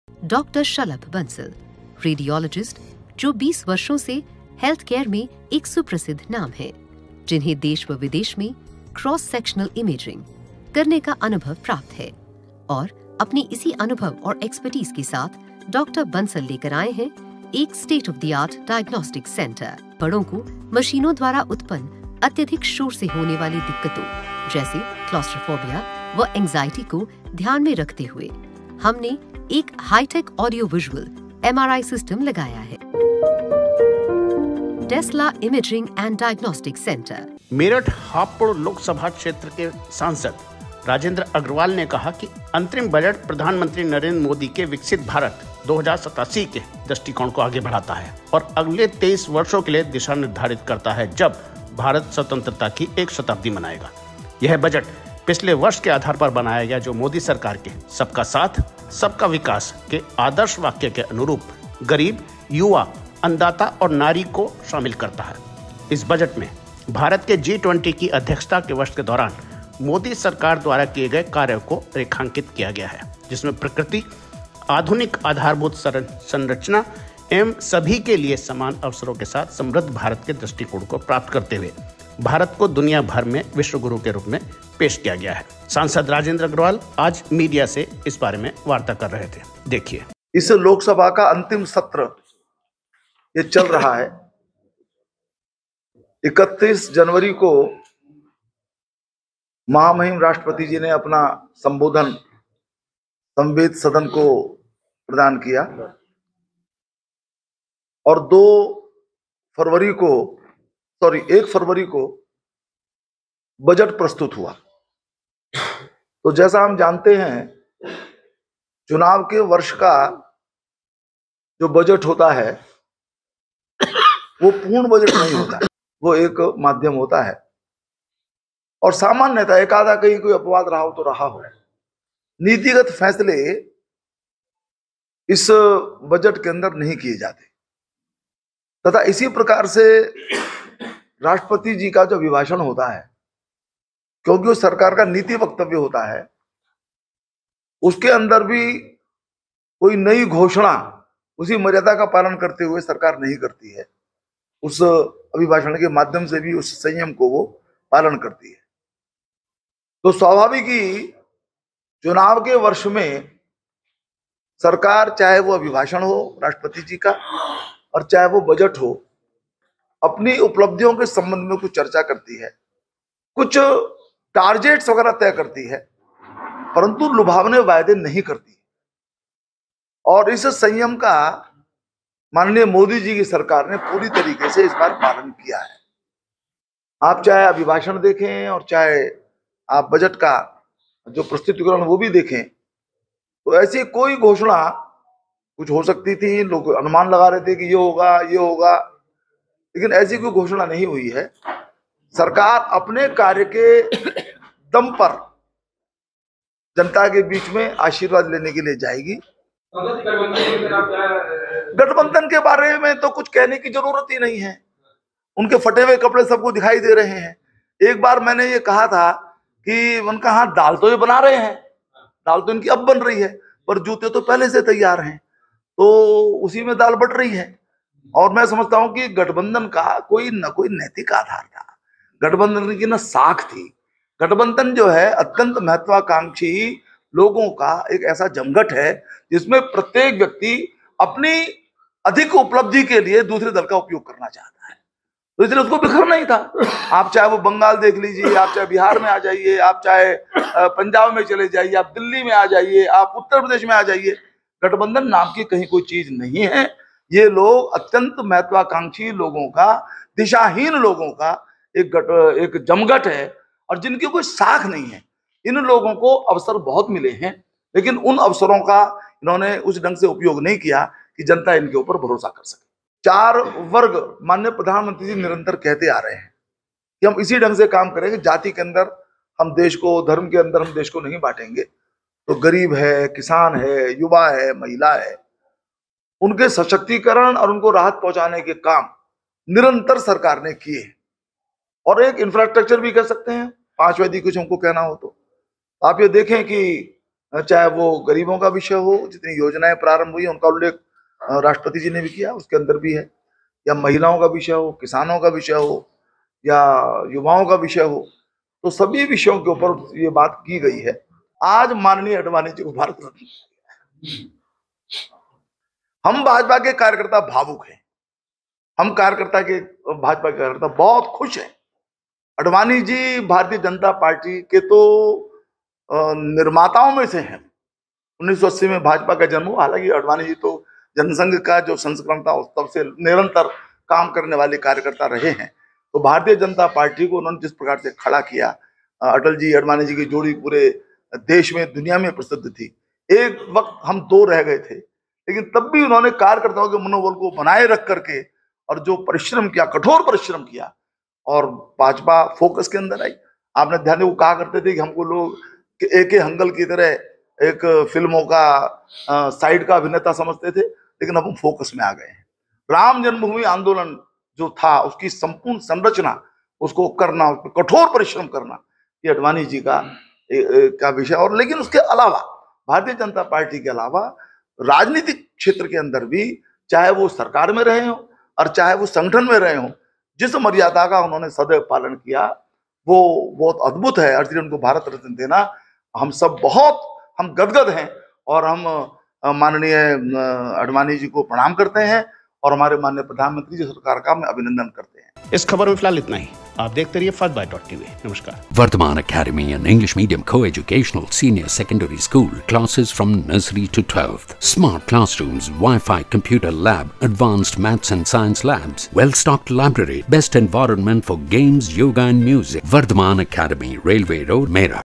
अंतरिम बजट ने भारत को विश्व गुरु के रूप में पेश किया-राजेंद्र अग्रवाल - First Byte Tv
मेरठ हापुड़ लोकसभा के सांसद ने की प्रेस वार्ता
अंतरिम बजट विकसित भारत का दृष्टिकोण
23 वर्षों के लिये दिशा निर्धारित करता है यह बजट
सबका साध, सबका विकास के अनुरूप है बजट-राजेंद्र
भारत को विश्व गुरू के रूप में पेश है किया गया